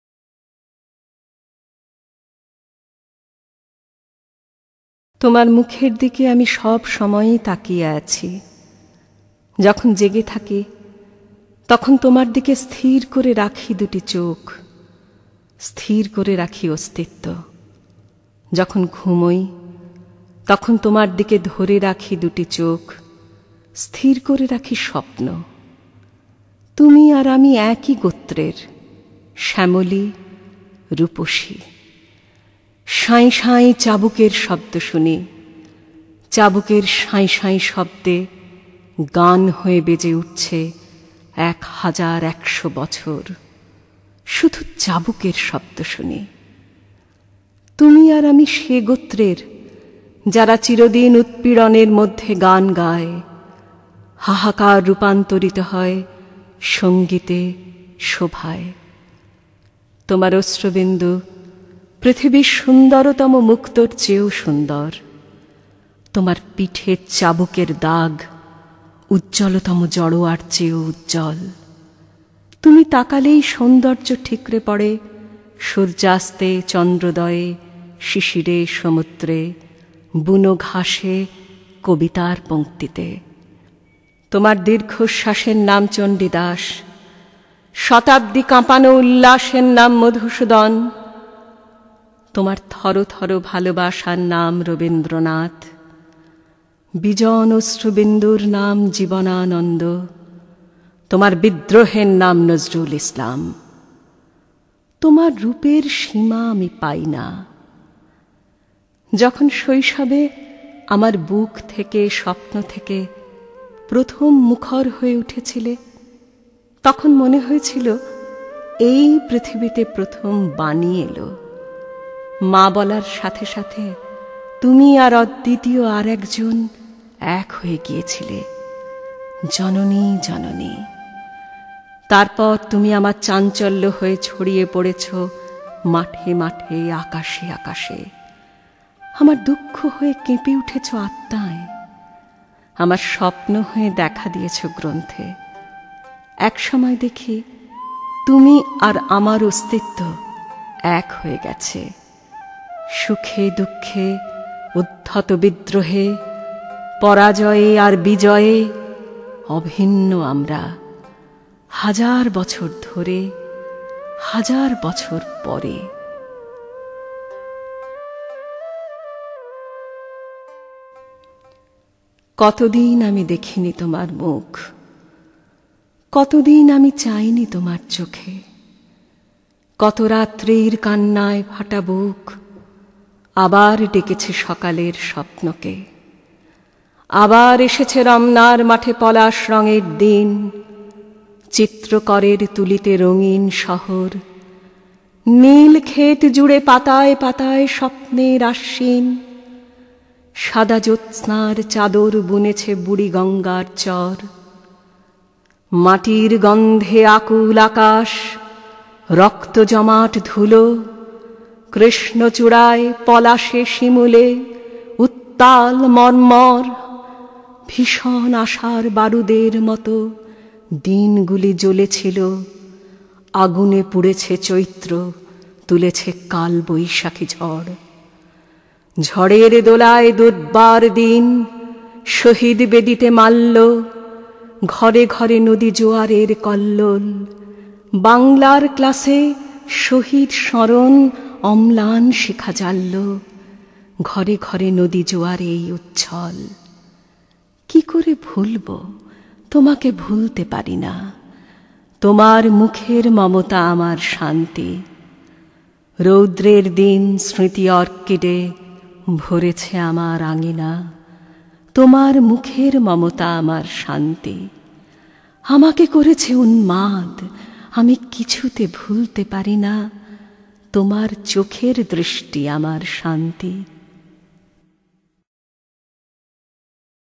Tomar Mukher Dike by Humayun Azad
Recitation:- Ratna Mitra
Audio:- Banglar Kato Katha
Company:- Cozmic Harmony
Picture Courtesy:- Saikat Kundu
Video Compilation:- Arna Seal